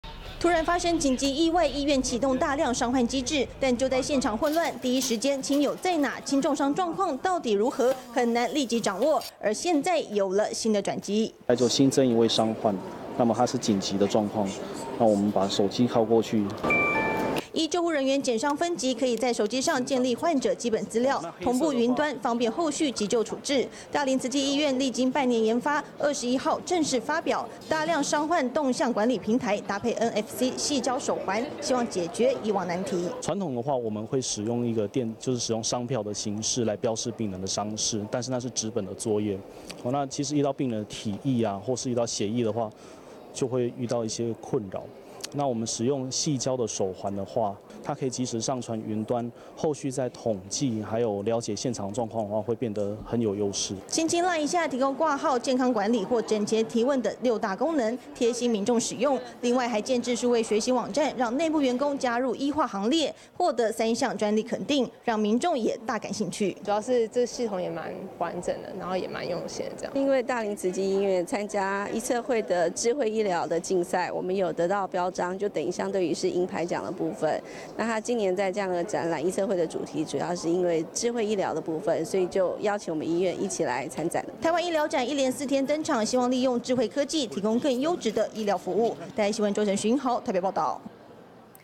台灣醫療展 最新發明提升醫療品質
台灣國際醫療展，今天上午登場，超過四百家廠商參與，今年以智慧醫療為主題，大林慈濟醫院正式發表「大量傷患識別手環」，能夠統整救災現場傷患資訊，即時上傳雲端，增加救護效率。

突然發生緊急意外，醫院啟動大量傷患機制，但救災現場混亂，第一時間，親友在哪，輕重傷狀況到底如何，很難立即掌握。而現在有了新的轉機。

依救護人員檢傷分級，可以在手機上建立患者基本資料，同步雲端，方便後續急救處置，大林慈濟醫院歷經半年研發，21號正式發表「大量傷患動向管理平台」，搭配NFC矽膠手環，希望解決以往難題。

大林慈濟醫院急診部醫師 鄭伯良：「傳統的話，我們會使用傷票的形式來標示患者傷勢，但是那是紙本作業，其實遇到病人體液或血液，就會遇到一些困難，我們使用矽膠的手環的話，那可以即時上傳雲端，後續在統計，還有了解現場狀況，會變得很有優勢。」

輕輕LINE一下，提供掛號、健康管理，或診前提問等六大功能，貼心民眾使用。另外還建置數位學習網站，讓內部員工加入E化行列，獲得三項專利肯定，讓民眾都大感興趣。

民眾 彭小姐：「主要是這系統滿完整的，也滿用心的這樣。」

大林慈濟醫院教學部主任 陳弘美：「因為大林慈濟醫院參加醫策會的醫療的競賽，我們有得到標章，相對於是銀牌獎的部分，今年在這樣的展覽，醫策會的主題智慧醫療的部分，所以就邀請我們醫院一起來參展。」

台灣醫療展，一連四天登場，希望利用智慧科技，提供更優質的醫療服務。

周怡呈 徐英豪 台北報導